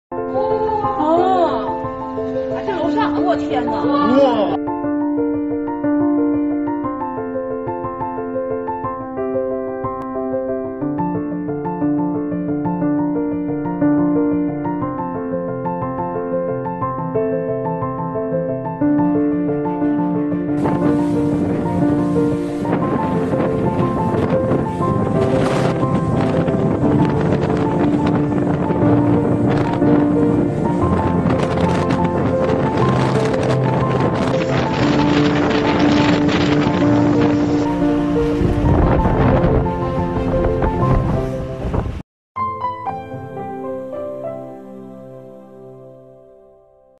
台风“美莎克”袭珲春 掀飞屋顶拔起树
9月3日下午，今年第9号台风“美莎克”移入吉林省，在中俄朝三国交界地带的珲春市受到较大影响。珲春市一楼房屋顶部分构造被大风掀翻。在敬信镇，大风将大树刮倒。同时，河水水位也在上升。